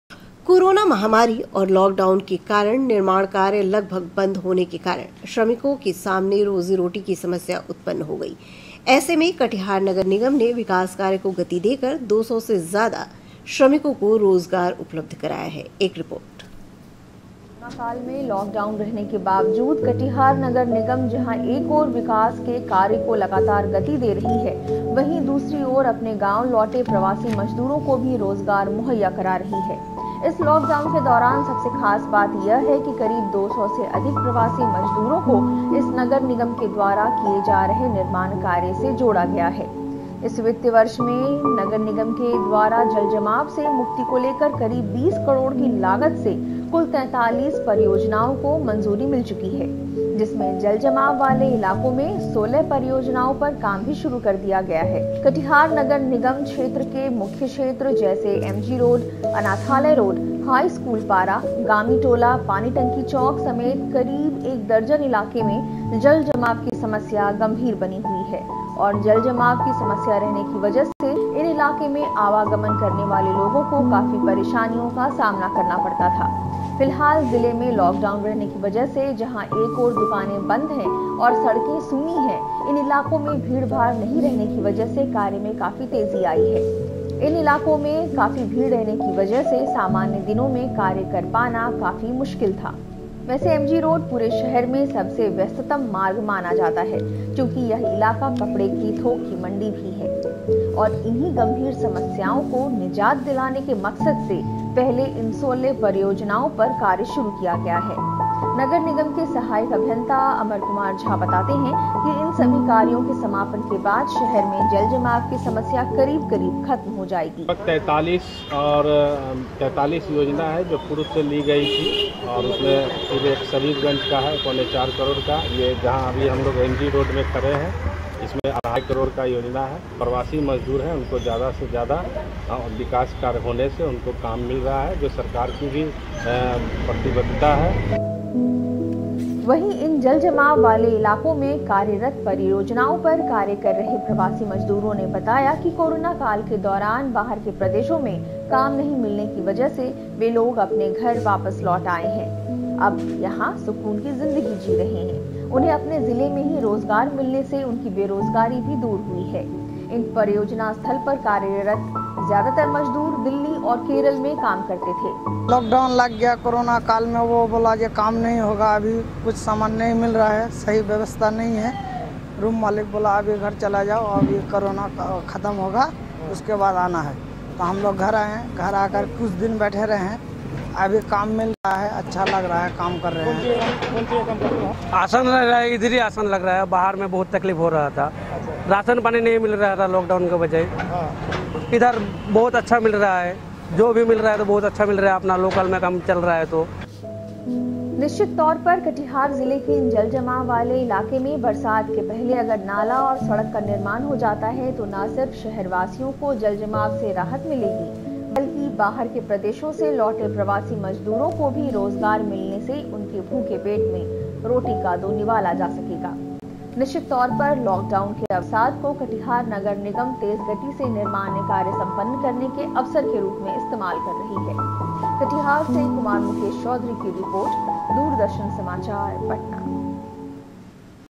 कटिहार नगर निगम ने विकास कार्य को गति देकर दो सौ से ज्यादा श्रमिकों को रोजगार उपलब्ध कराया
कोरोना महामारी और लॉकडाउन के कारण निर्माण कार्य लगभग बंद  होने के कारण श्रमिकों के सामने रोजी-रोटी की समस्या उत्पन्न हो गयी है। ऐसे में कटिहार नगर निगम ने विकास कार्य को गति देकर दो सौ से ज्यादा श्रमिकों को रोजगार उपलब्ध कराया है। एक रिपोर्ट---